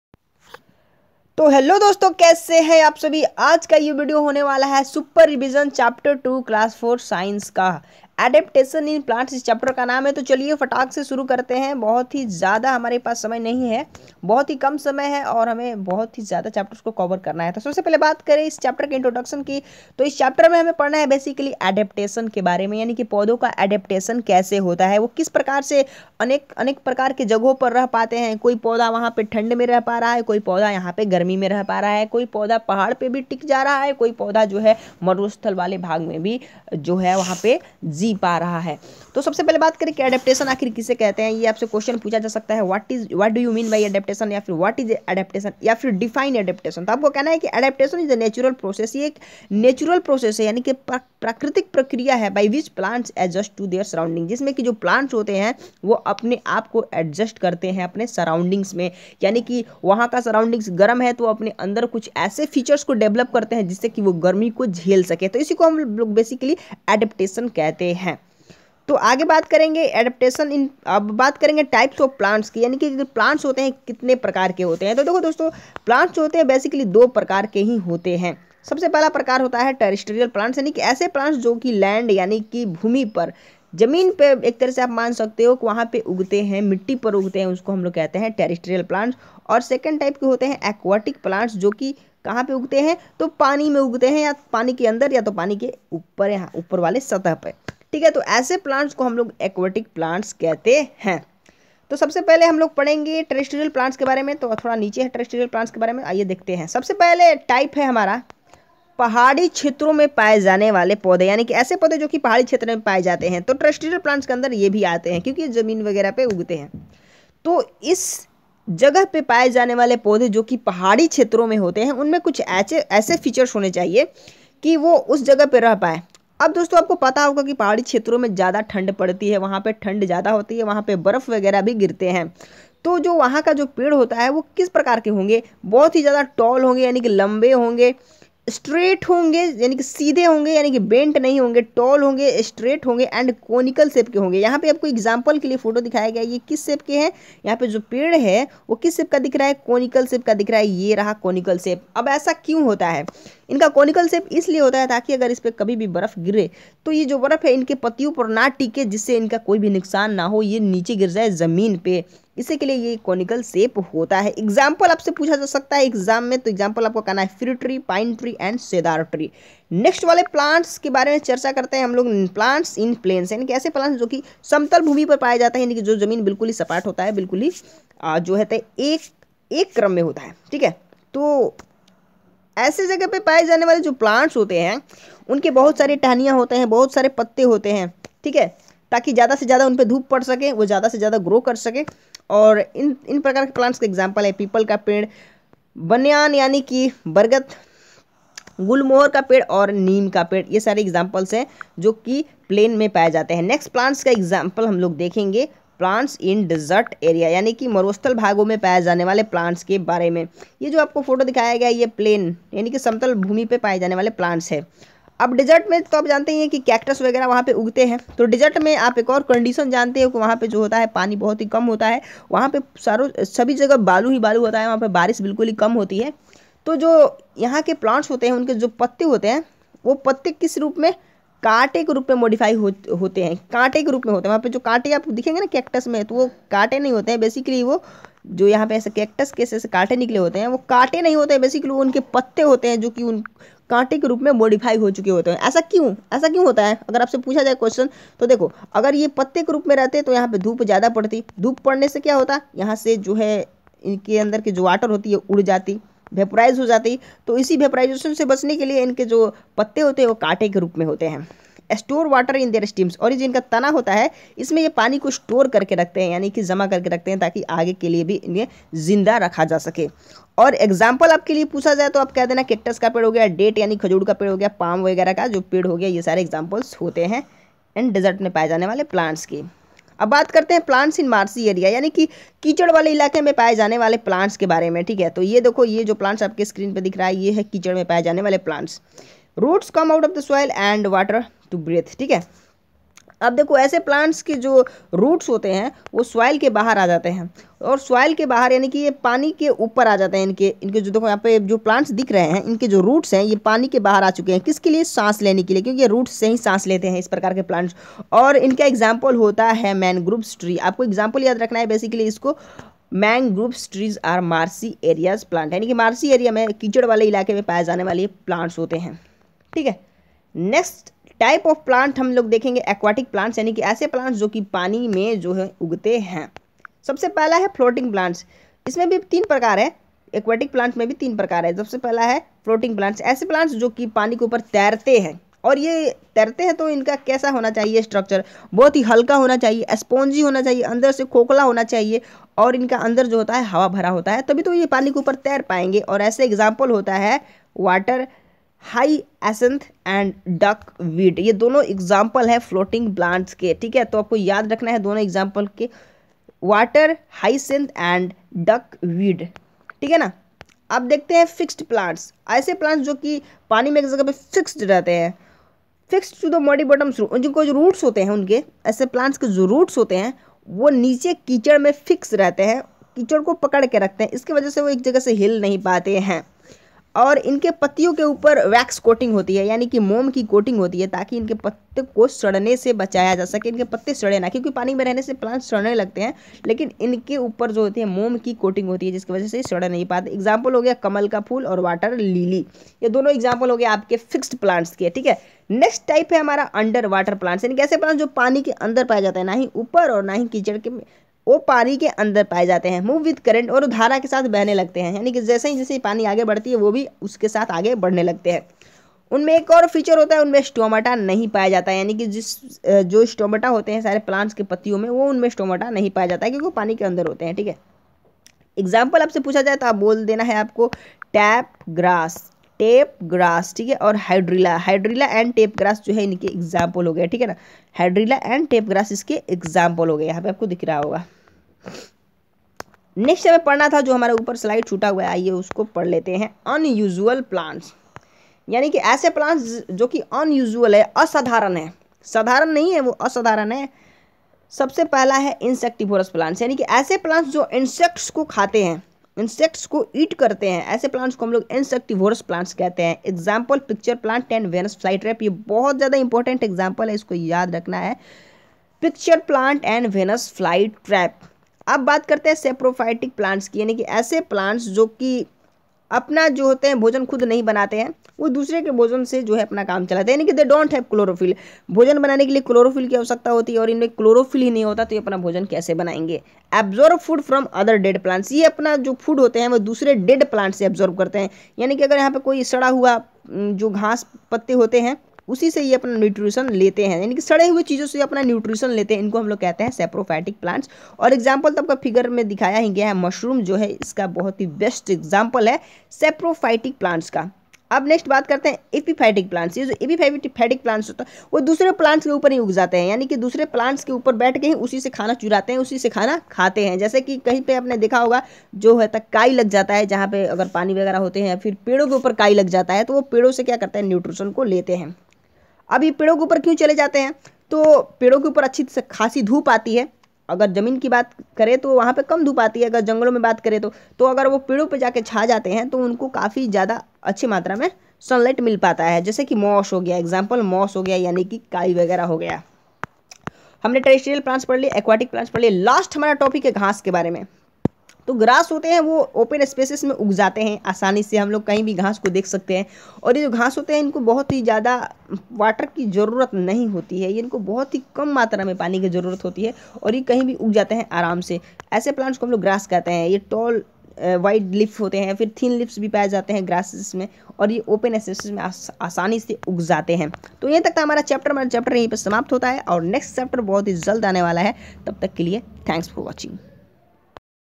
Adaptation in Plants Class 4 science Chapter 2 Revision
class 4 science exam video
class 4 science chapter 2 one shot

adaptation in Plants one shot revision.

topics
adaptation,
types of plants,
terrestrial plants,
aquatic plants,
unusual plants,
insectivores plants,
grasses,